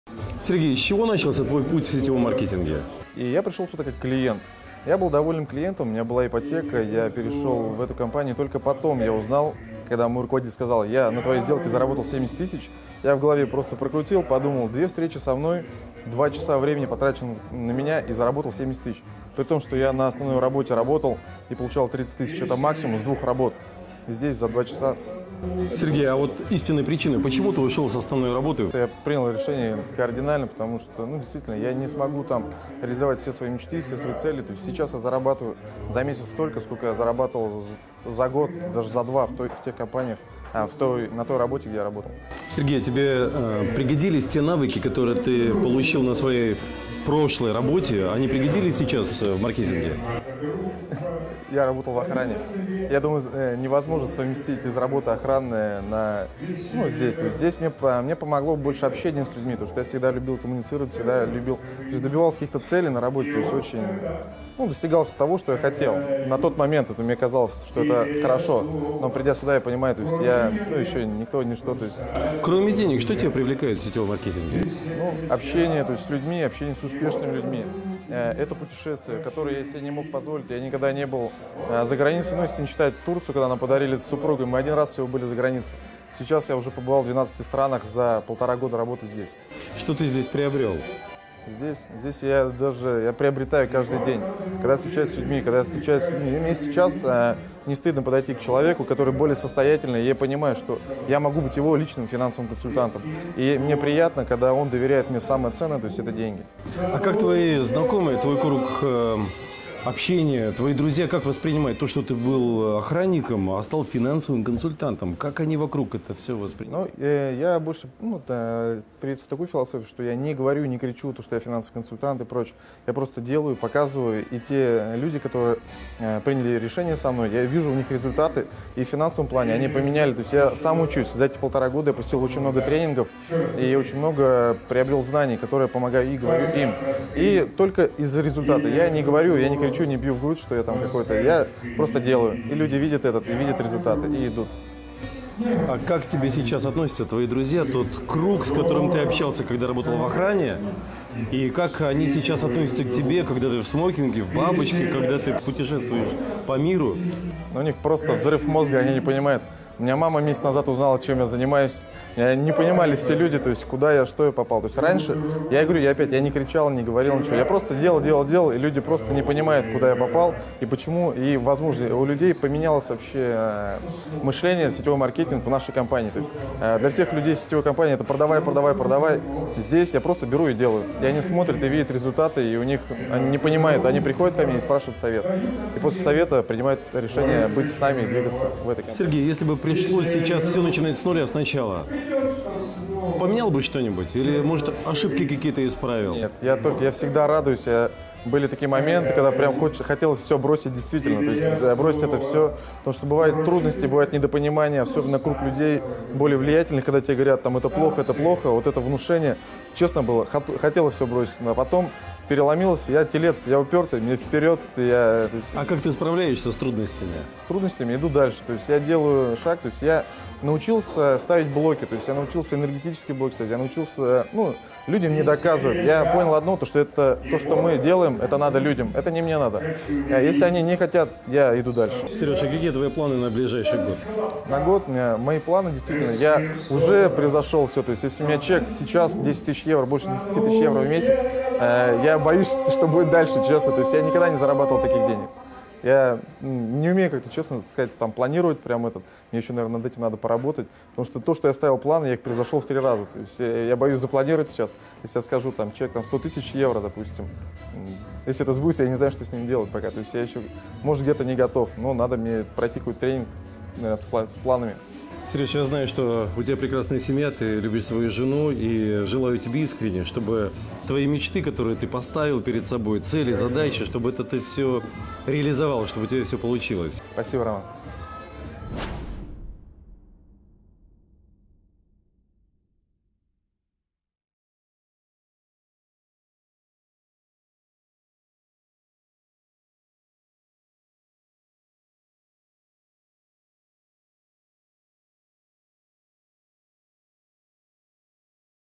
КАК УВЕЛИЧИТЬ ДОХОД В 20 РАЗ реальная история успеха в сетевом маркетинге
Сегодня я хочу представить вам интервью с Сергеем Санниковым, одним и лидеров команды LifeisGood. Всего за полтора года Сергей уже добился такиех результатов, о которых и не мечтал. Его доходы выросли в 20 раз! А ведь он до работы в холдинге не имел никакого отношения к бизнесу, к продажам. Вот чего можно добиться, когда есть сильное желание и четкая цель